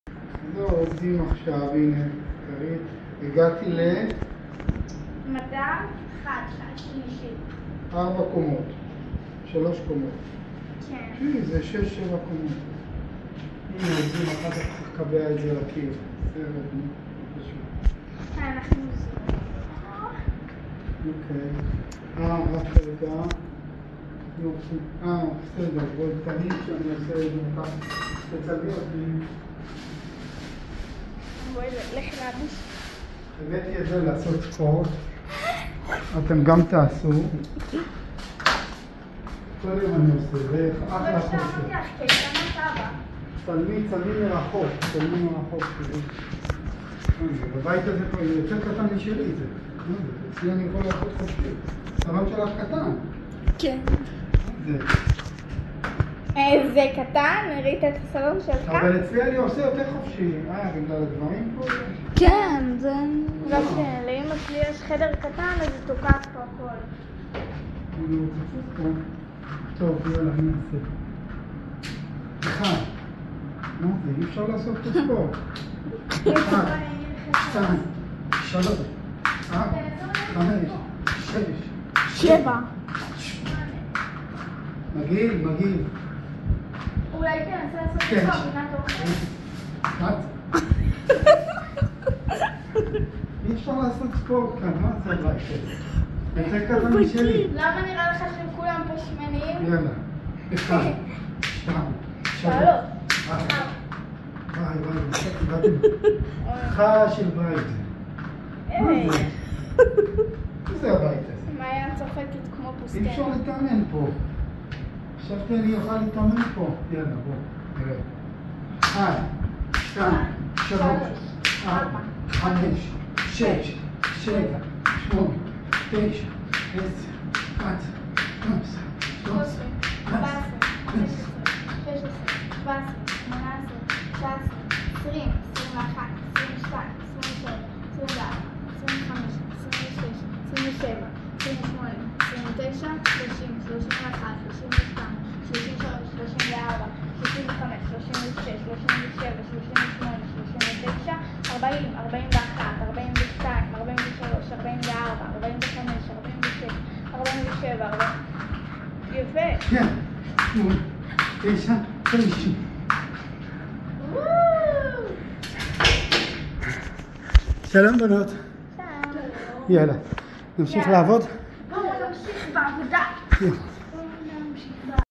אריה טייב ושירה טייב בבית החדש
אריה ושירה טייב משחקים